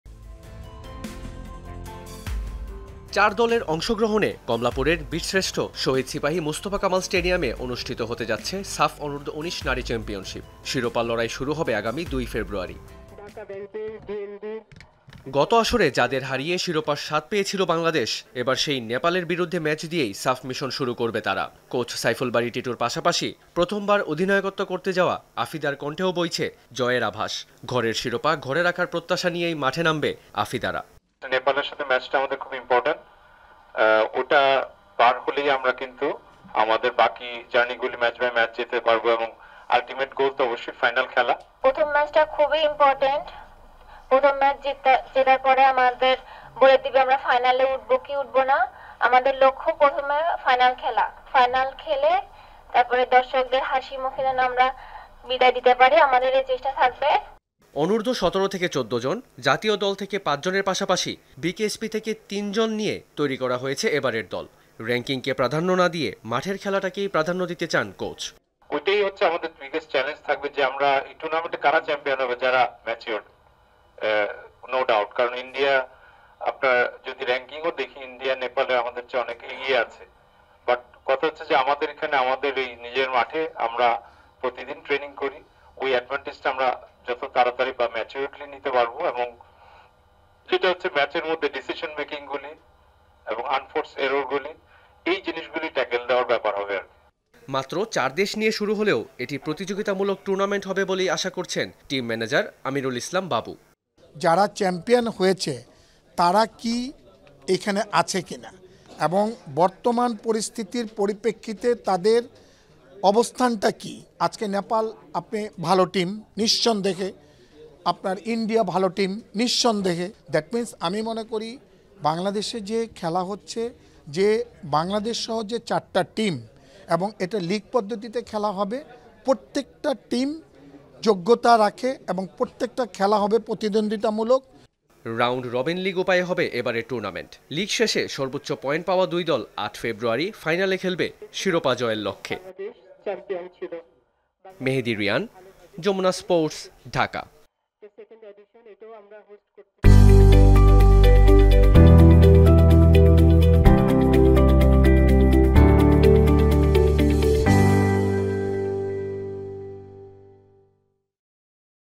মুকুট ধরে রাখার মিশনে সাফে নামছে বাংলাদেশের মেয়েরা | SAFF | Jamuna Sports
আগামী ২ ফেব্রুয়ারি কমলাপুরের বীরশ্রেষ্ঠ শহীদ সিপাহী মোস্তফা কামাল স্টেডিয়ামে শুরু হবে সাফ অনূর্ধ্ব ১৯ নারী চ্যাম্পিয়নশীপ। যেখানে গতবারের ফাইনালিস্ট নেপালের বিরুদ্ধে ম্যাচ দিয়ে মিশন শুরু করবে বাংলাদেশের মেয়েরা।  বছরের প্রথম টুর্নামেন্টে শিরোপা ধরে রাখার প্রত্যাশা কোচ ও অধিনায়ক দু'জনেরই। টুর্নামেন্টের বাকি দুই দেশ- ভারত ও ভুটান।

Address:
Bangladesh